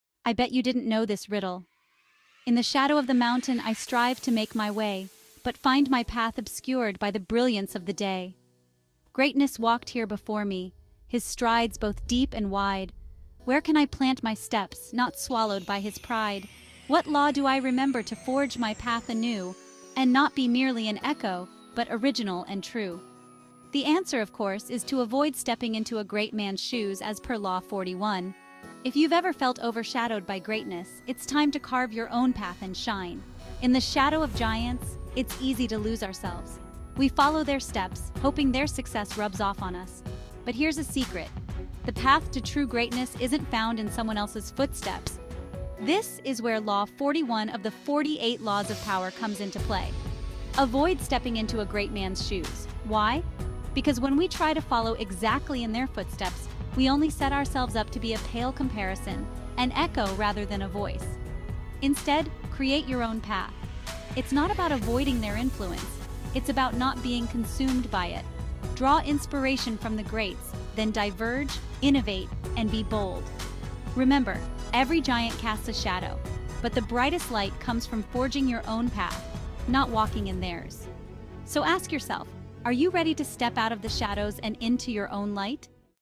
Power Law 41 Explained
Law 41 of Robert Greene's "48 Laws of Power" is titled "Avoid Stepping into a Great Man's Shoes." This law advises against following directly in the footsteps of a highly successful predecessor. Greene argues that if you take on a role or position that was previously held by someone widely admired and successful, you are likely to suffer by comparison. No matter how competent you are, your achievements may seem diminished next to the towering legacy left by the previous incumbent.

The law suggests that it is better to forge your own path or find a new niche where you can set your own standards and be judged on your own terms. If you do find yourself in a position previously held by a "great man," Greene advises making your own mark by transforming the role to suit your unique abilities and style, thereby establishing a distinct identity that can be appreciated on its own merits.